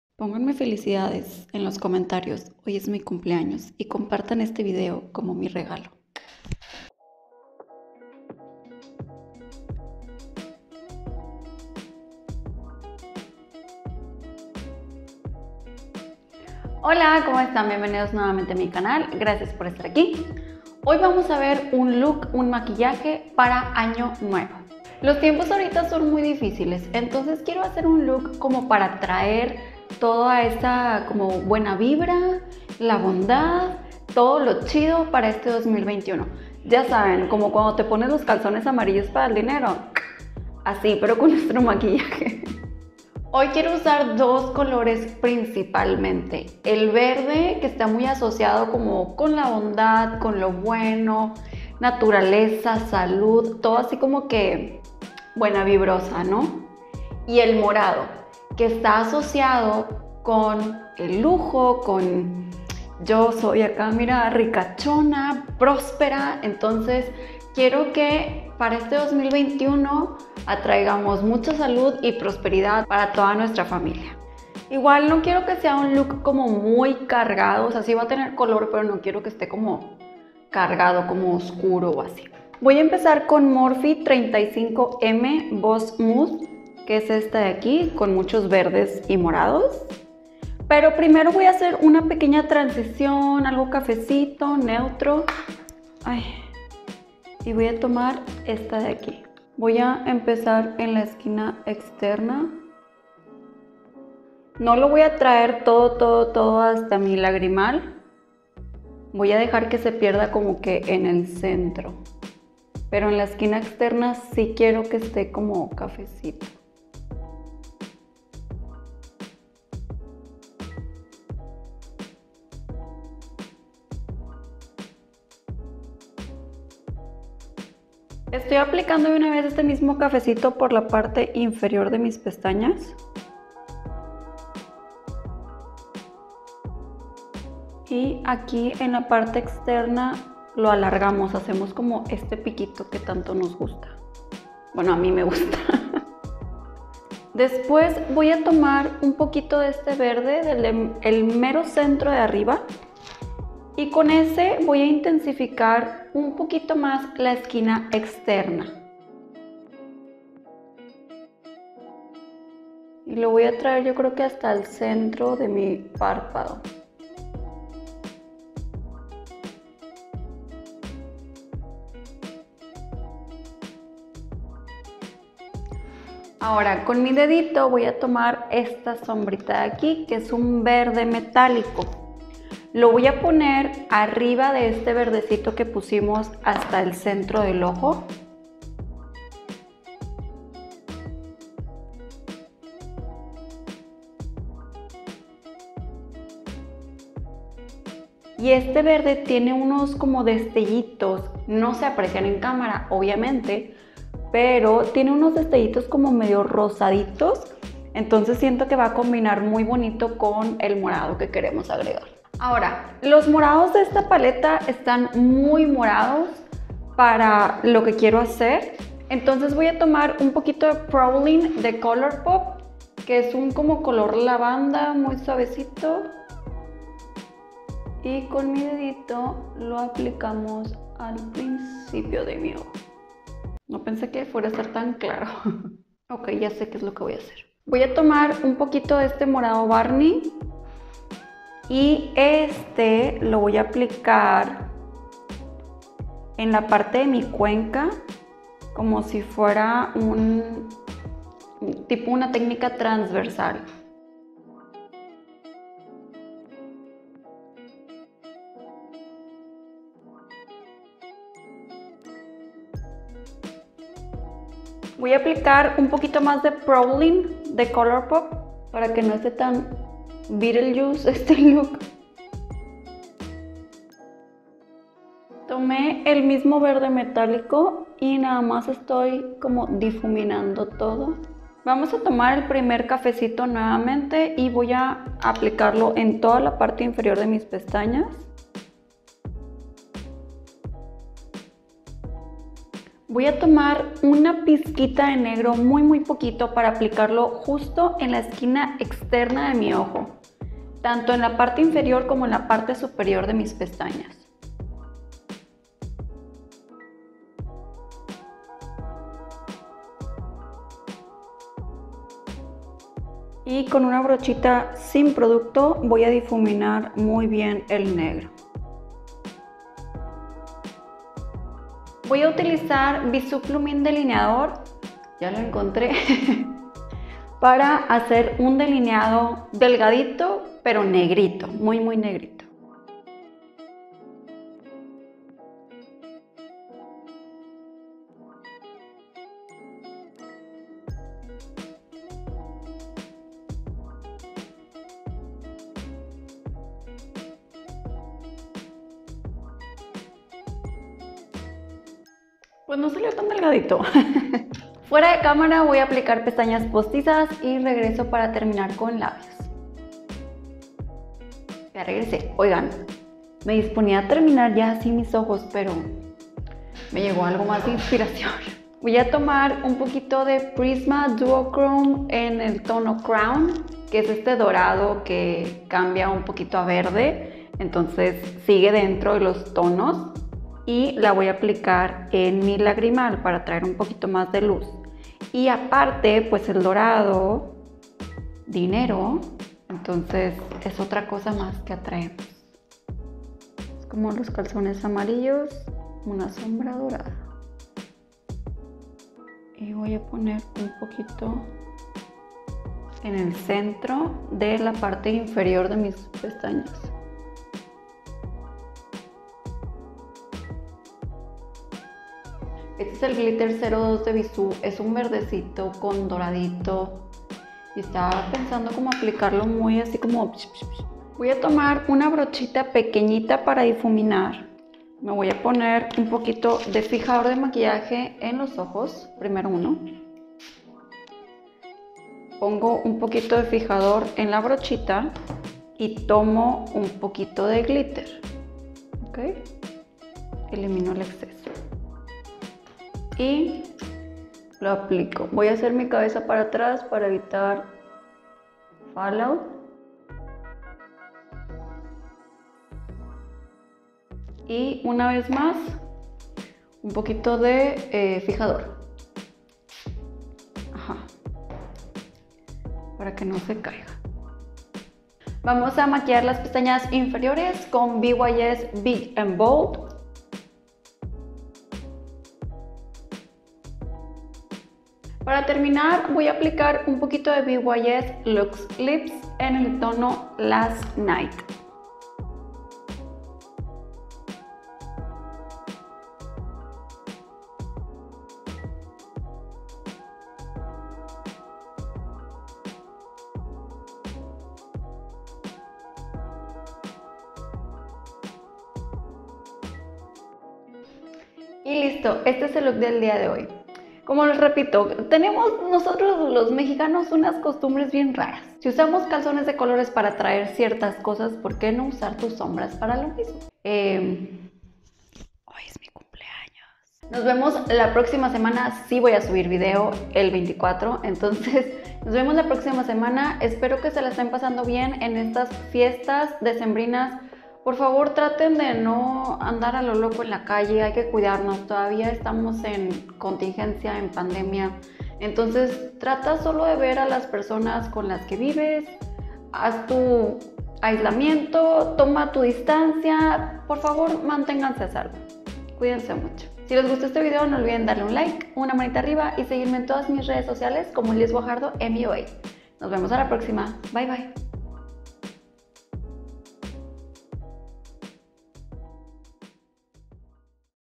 Maquillaje para despedir el 2020 | Atraer salud y prosperidad | Liz Guajardo MUA
Quiero empezar una nueva tradición a nuestra cultura curiosa Mexicana. Ahora aparte de esconderte abajo de la mesa para conseguir pareja, que tal usar maquillaje de color según lo que queramos atraer?

- Productos utilizados - 🤓
Morphe Brushes
35M

BYS Cosmetics
Big and bold
Luxe lips - last night

Prisma Cosmetics
Duo chrome - crown
Mink eyelashes - 02

Bissú Cosmetics
Plumín delineador - negro
Glitter - 02